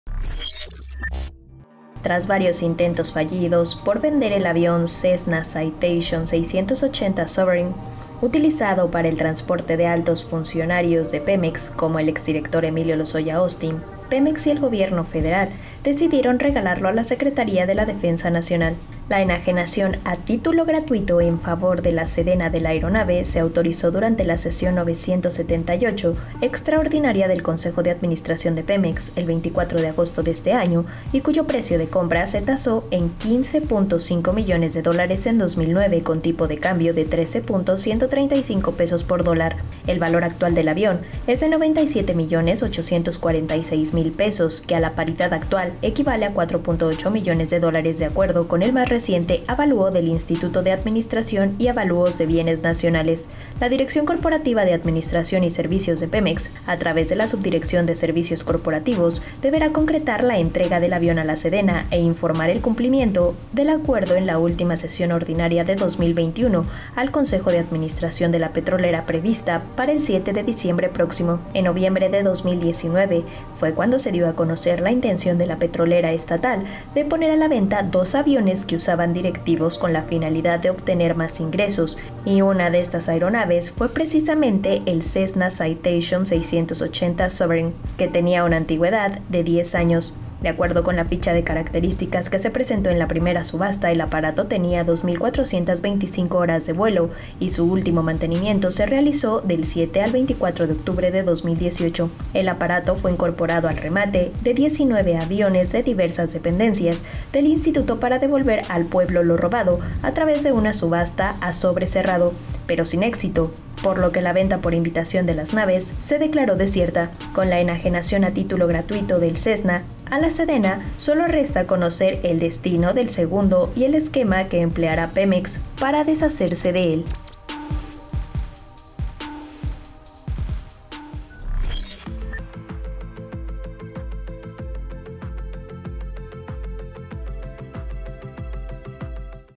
Pemex regala a Sedena el avión que usaba Lozoya
Tras varios intentos por venderlas, una de las aeronaves empleadas para trasladar ejecutivos se entregó al Ejército